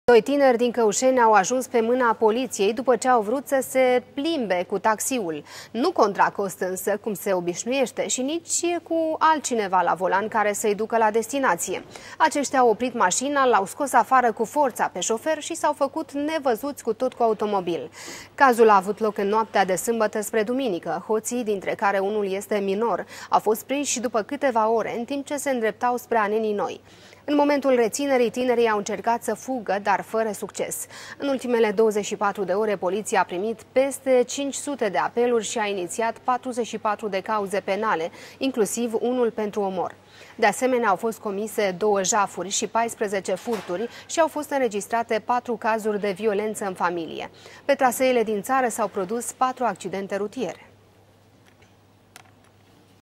Taxi furat la Căuşeni! Aventura a doi tineri s-a încheiat la secţia de poliţie
Doi tineri din Căuşeni au ajuns pe mâna poliţiei după ce au vrut să se plimbe cu taxiul. Nu contra cost însă, cum se obinuieşte, şi nici cu altcineva la volan, care să-i ducă la destinaţie. Aceştia au oprit maşina, l-au scos afară cu forţa pe şofer şi s-au făcut nevăzuţi cu tot cu automobil.

Cazul a avut loc în noaptea de sâmbătă spre duminică. Hoţii, dintre care unul este minor, au fost prinşi după câteva ore, în timp ce se îndreptau spre Anenii Noi. În momentul reţinerii, tinerii au încercat să fugă, dar fără succes.

În ultimele 24 de ore, poliţia a primit peste 500 apeluri şi a iniţiat 44 de cauze penale, inclusiv unul pentru omor.

De asemenea, au fost comise două jafuri şi 14 furturi şi au fost înregistrate patru cazuri de violenţă în familie. Pe traseele din ţară s-au produs patru accidente rutiere.